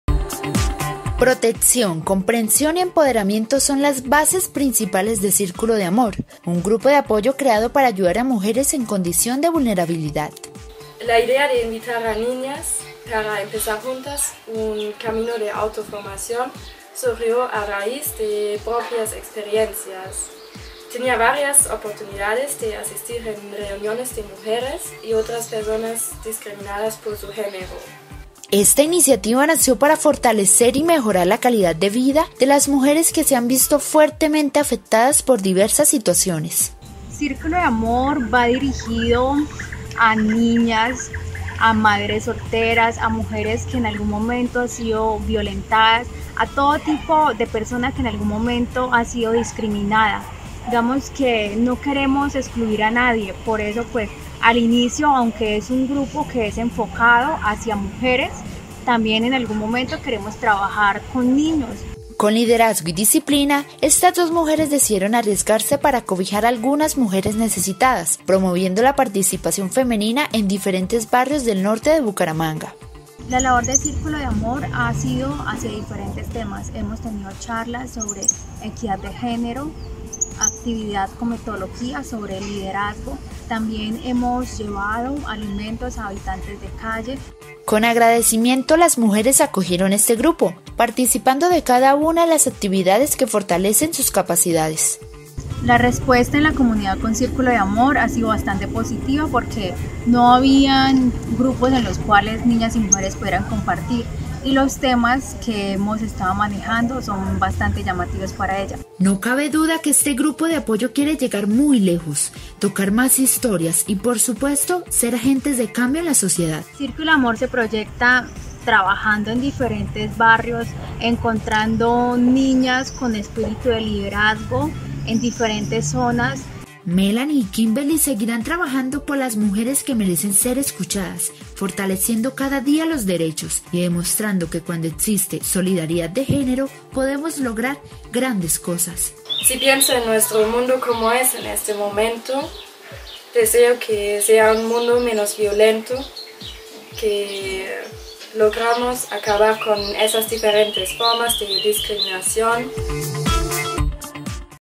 Grupo de apoyo Círculo de amor
Hoy hablamos de un grupo de apoyo llamado "Círculo De Amor" que busca el bienestar colectivo para los más necesitados.